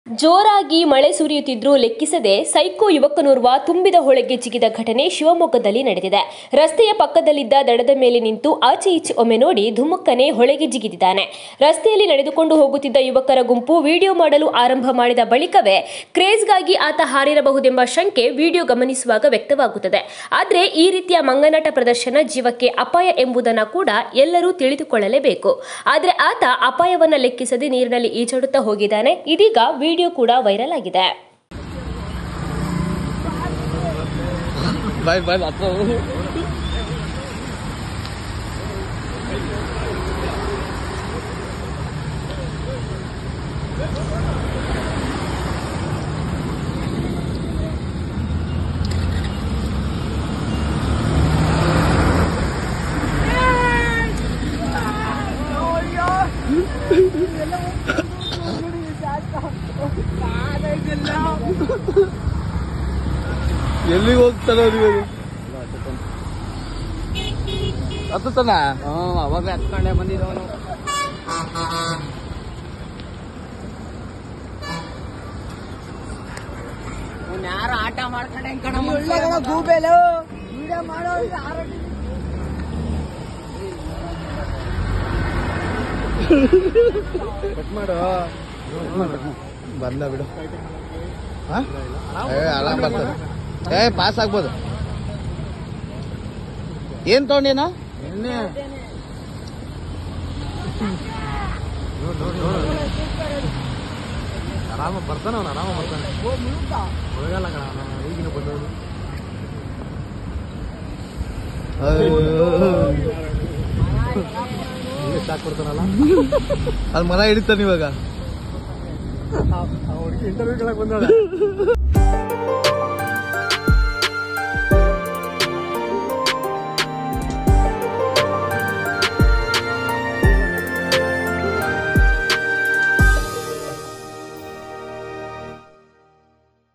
ಶಿವಮೊಗ್ಗದಲ್ಲಿ ತುಂಬಿದ ಹೊಳೆಗೆ ಜಿಗಿದ ಸೈಕೋ ಯುವಕ | News Kannada
ಶಿವಮೊಗ್ಗದಲ್ಲಿ ತುಂಬಿದ ಹೊಳೆಗೆ ಜಿಗಿದ ಸೈಕೋ ಯುವಕ